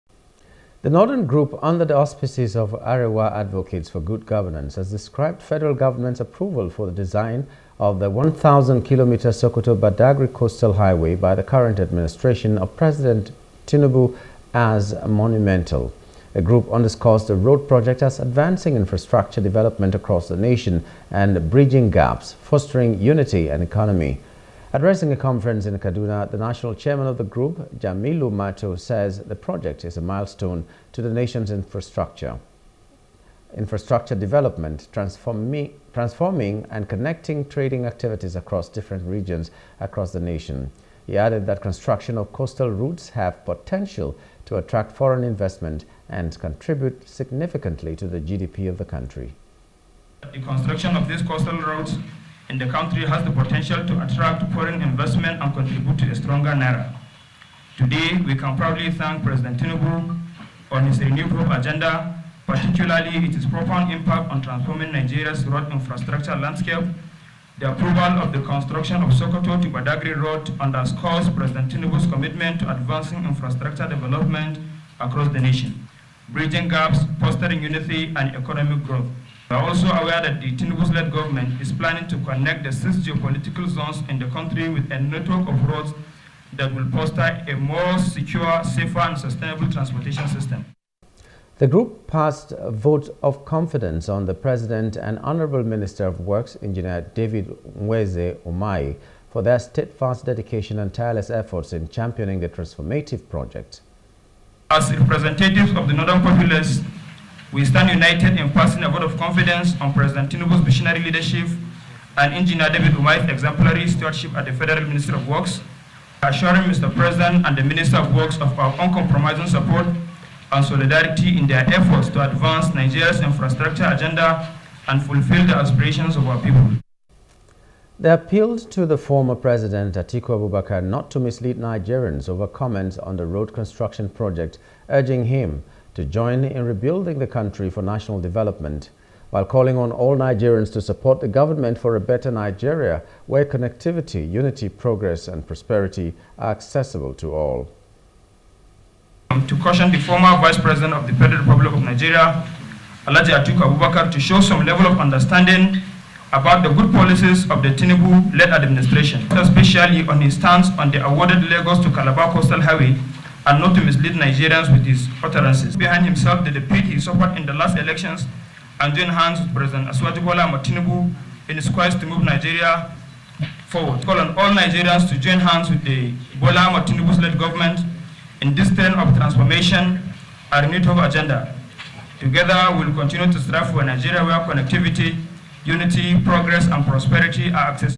SOKOTO BADAGRY COASTAL HIGHWAY NORTHERN GROUP LAUDS F G ’S APPROVAL
The Northern group under the auspices of Arewa Advocates for Good Governance has described federal government approval for the design of the 1000-kilometre Sokoto-Badagry Coastal Highway by the current administration of president Tinubu as monumental.